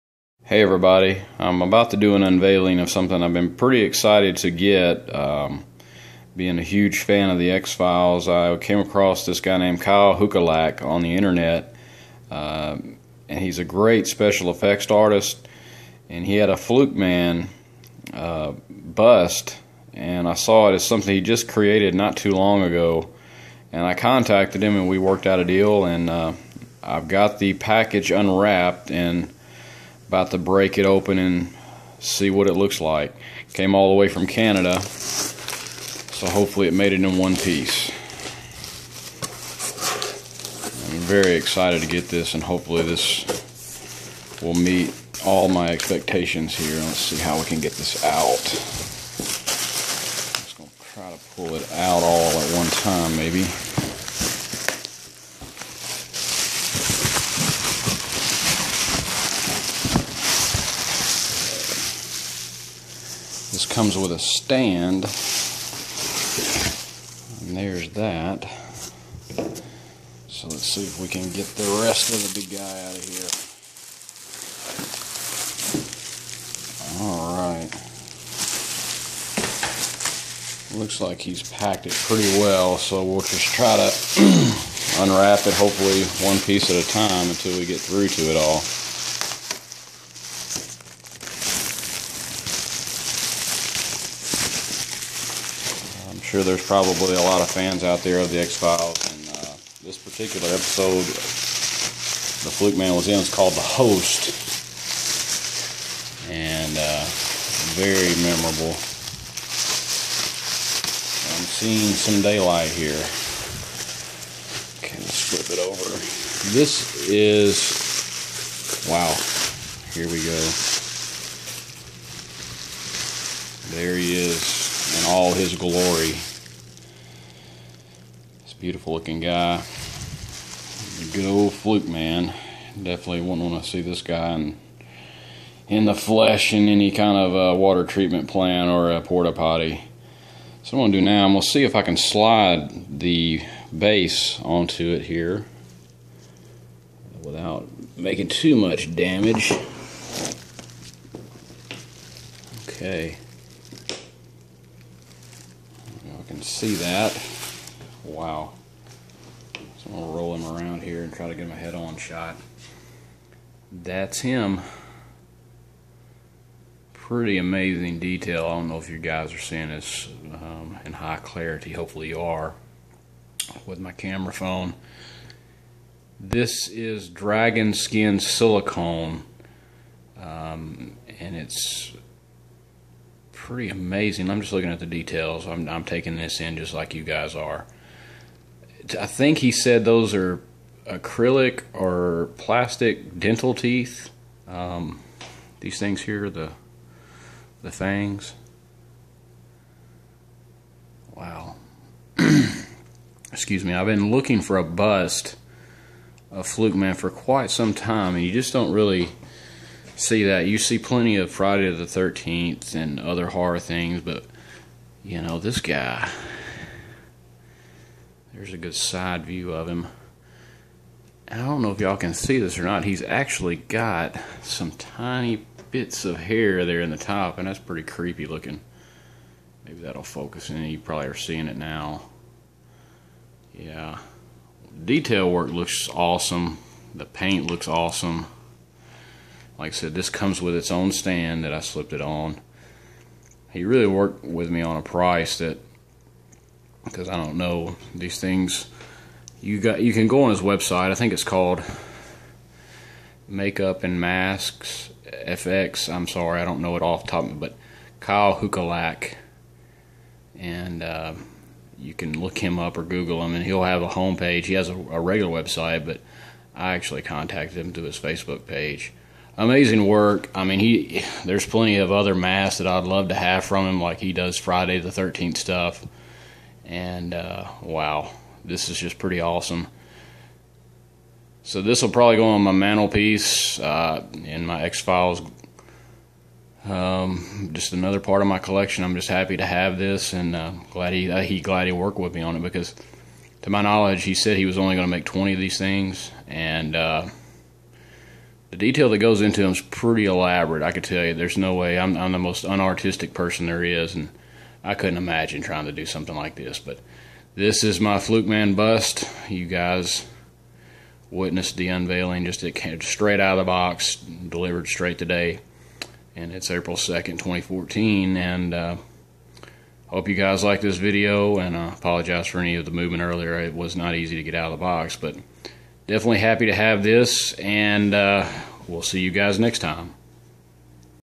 The X Files Flukeman Bust unboxing
Arrival of Flukeman Bust by artist Kyle Huculak